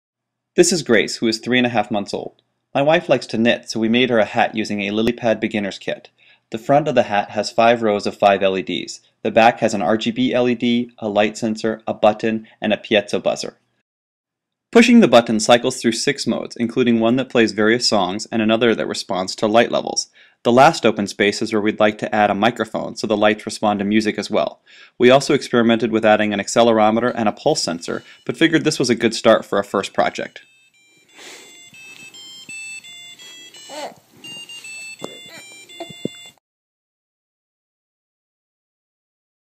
GraceHat (2012)
Last month I discovered Arduino when a friend gave me a SparkFun Inventor's Kit for my birthday. After building Oomlout's sample circuits, I started looking for other kits to play with, and ordered the Lilypad Beginner's Kit.

My wife likes to knit, so we made a hat for our 3-month-old daughter Grace.  The front of the hat has five rows of five LEDs. The back of the hat has an RGB LED, a button, a light sensor, and a piezo buzzer. Pushing the button cycles through six modes, including one that plays songs and another that responds to changing light levels.

The remaining space and pin is where we'd like to add a microphone so the lights respond to music as well. We experimented with adding an accelerometer and a pulse sensor, but figured this was a good start for a first project.

It's been a lot of fun to make and show people, so I'm grateful to SparkFun and Leah Buechley for the new hobby.